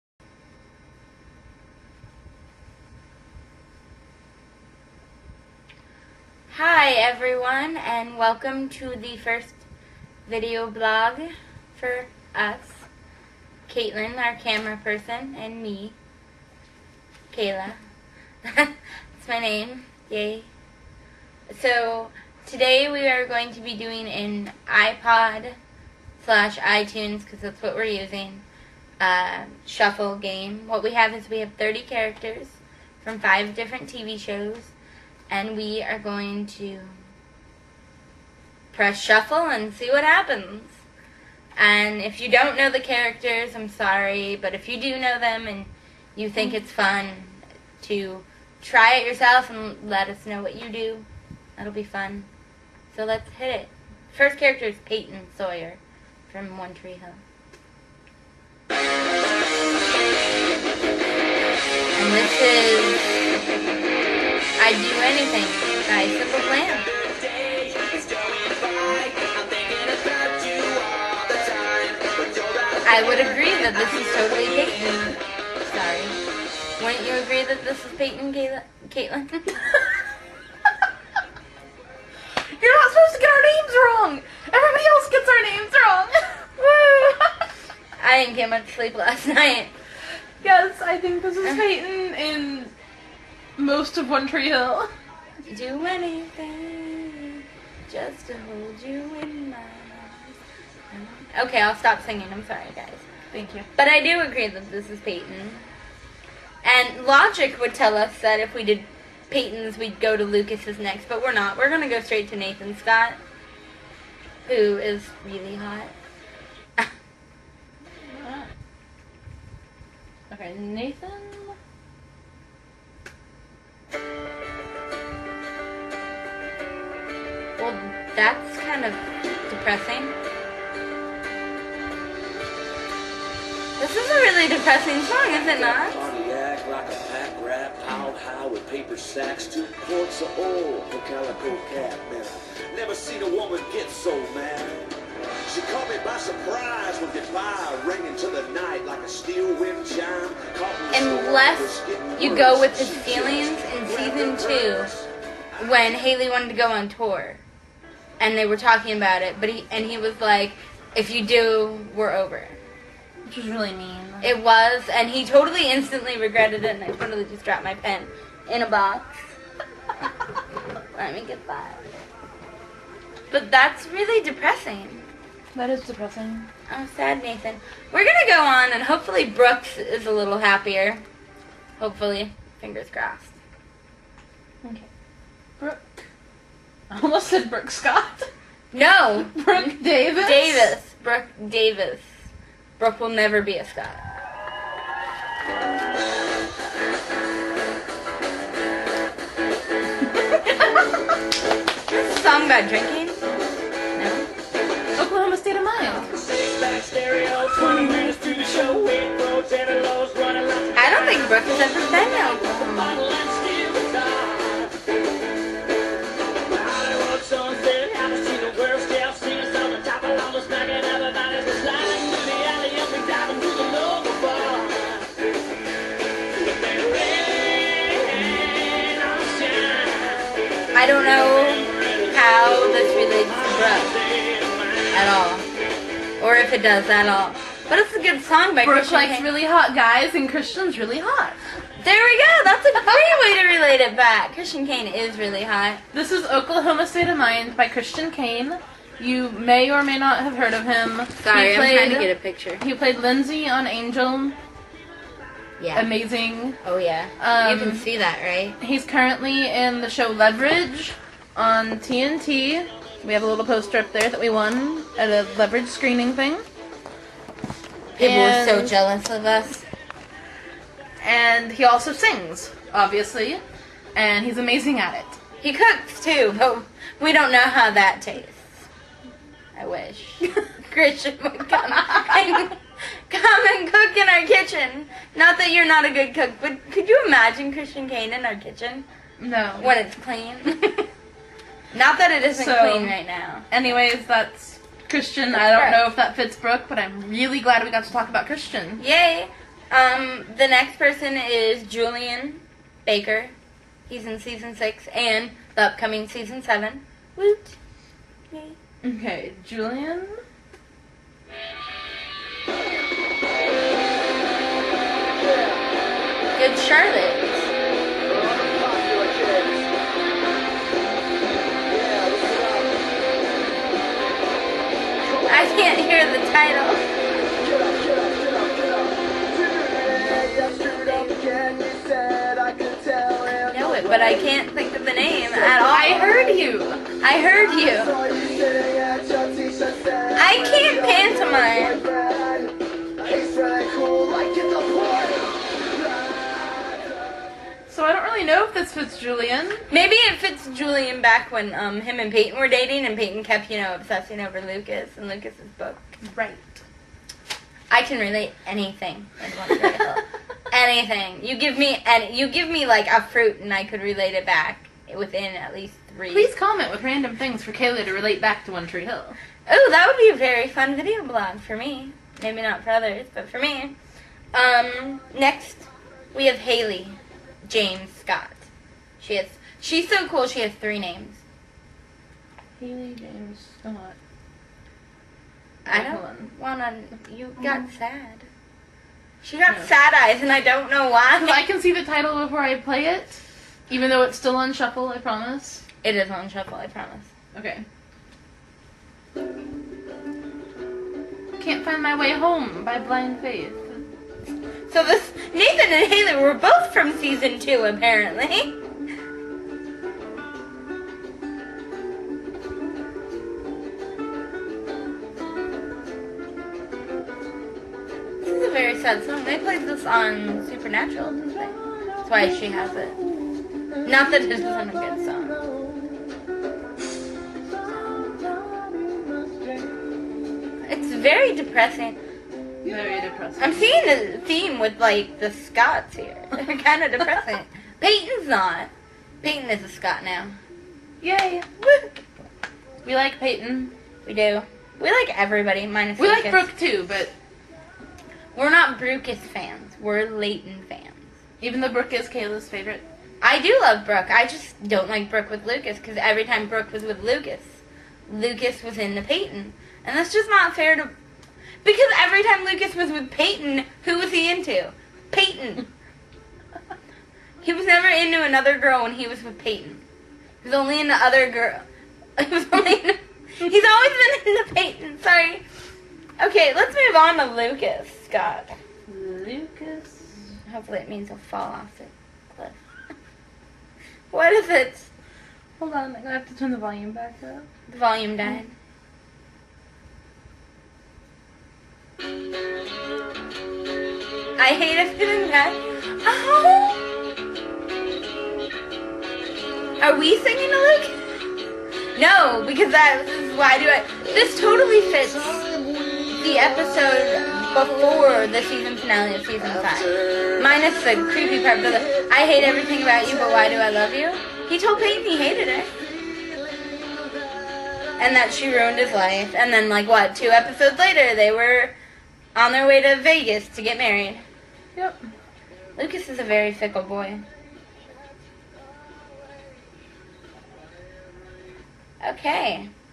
iPod Shuffle Vlog One Tree Hill
This is part one of a 6 part vlog that we did playing the 'Music shuffle' game with my iTunes, and characters from the shows One Tree Hill (this video), Leverage, True Blood, Supernatural, and Firefly.  We've split the vlog up based on show, with True Blood split into two sections because there were a lot of characters from that one.

Watch, have fun, enjoy, laugh at us, laugh at the songs the characters get, and post up some ideas for future vlogs!!

Peace.  Out.